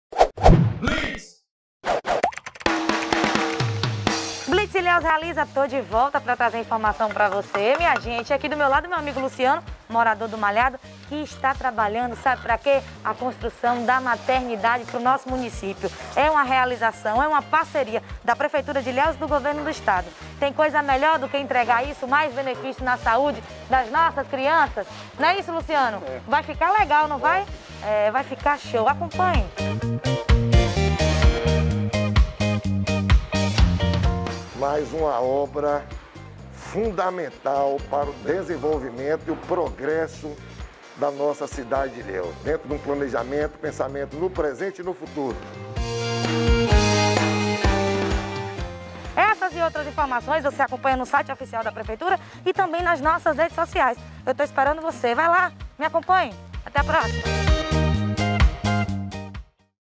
VISTORIA DA UNIDADE MATERNO-INFANTIL EM ILHÉUS (antigo Hospital Regional)
• A construção da unidade materno-infantil segue a todo vapor. O prefeito Mário Alexandre visitou a obra, que está com 40% de conclusão.
🏨 Serão 30 leitos de UTI e 90 clínicos, totalizando 120 leitos. A unidade também contemplará duas salas cirúrgicas.

▶️ Confira!